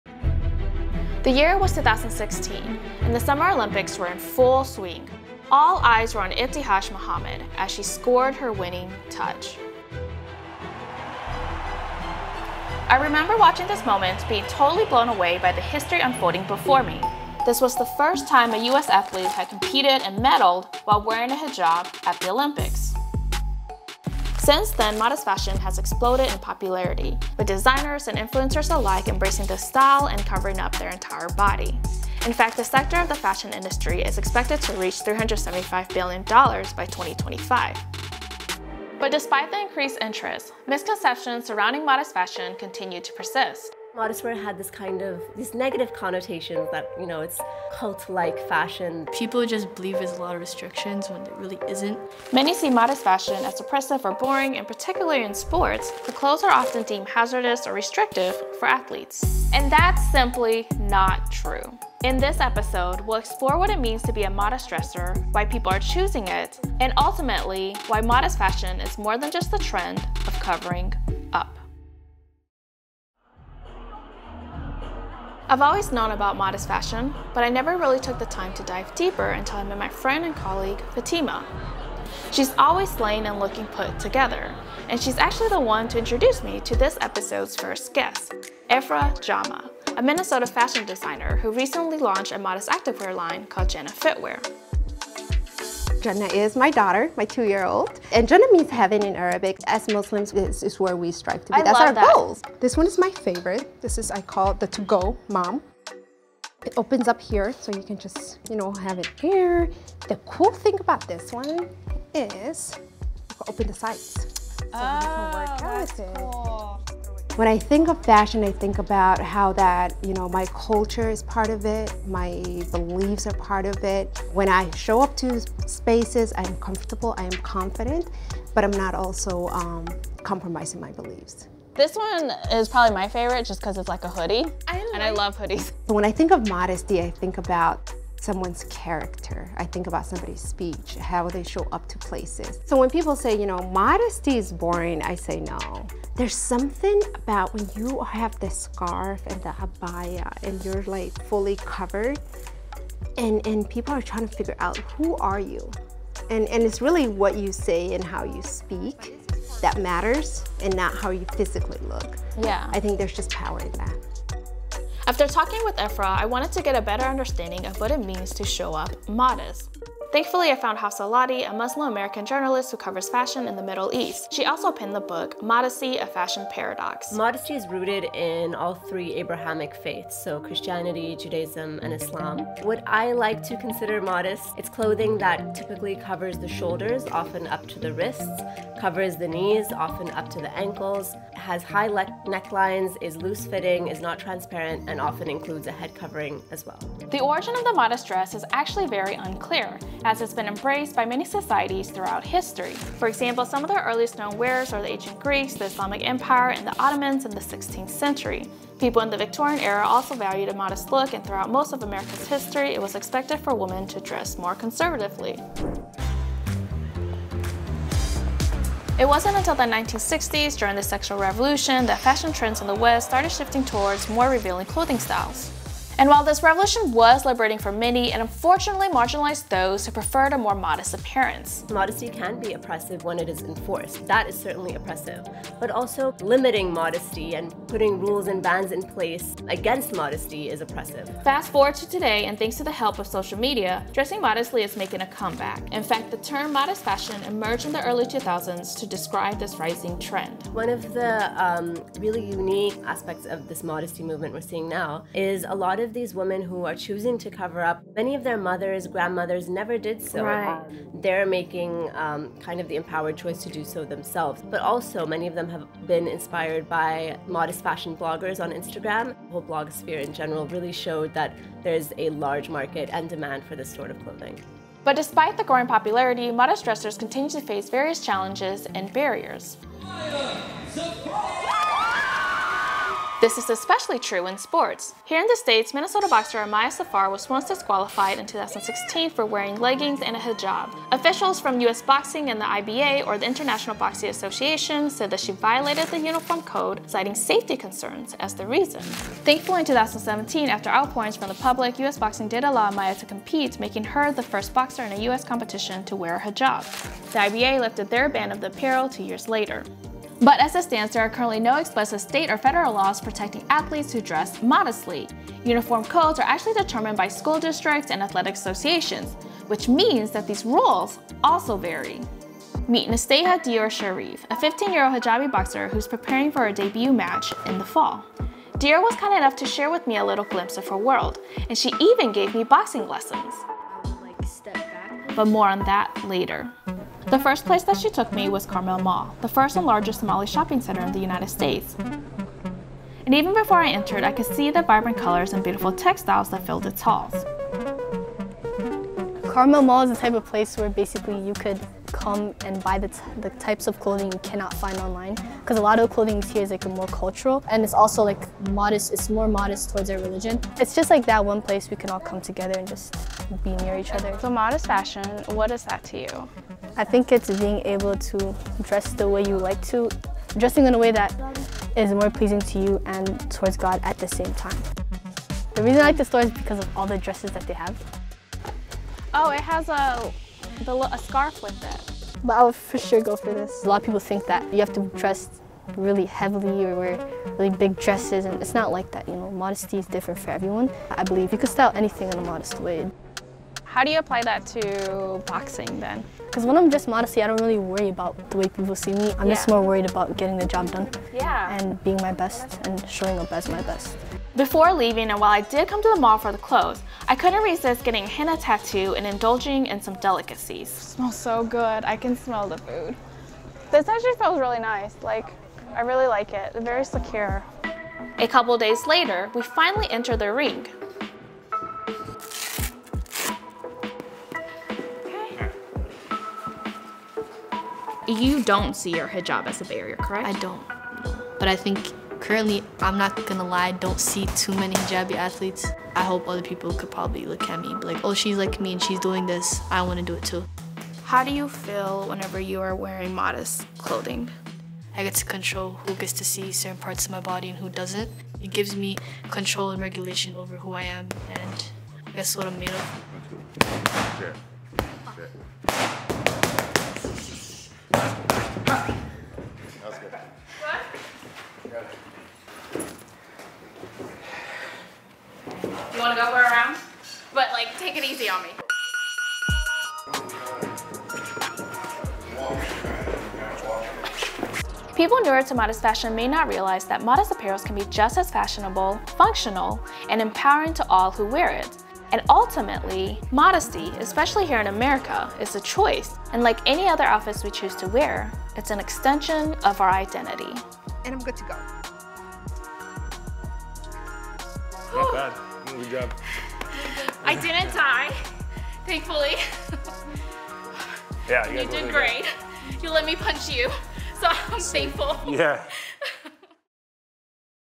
Why Modest Fashion Is Becoming More Popular | Worn Within
Modest fashion, though not new, has increased recognition and availability with major brands like Nike introducing their own modest clothing line. Yet, athletes still face challenges while competing in these garments. Join Susan as she dives deeper into the history of this clothing style and unravels its common misconceptions.

00:17 Ibtihaj Muhammad - First US Olympian to compete and medal while wearing a hijab
01:30 Ifrah Jama talks about her work as a MN Fashion Designer
02:50 What does Modest Fashion mean? A conversation with Hafsa Lodi
03:17 A brief history of Modest Fashion
04:40 Modest Fashion in sports
05:23 Conversation with boxer, Nasteha Sharif

This content is made possible by viewers like you. Support Twin Cities PBS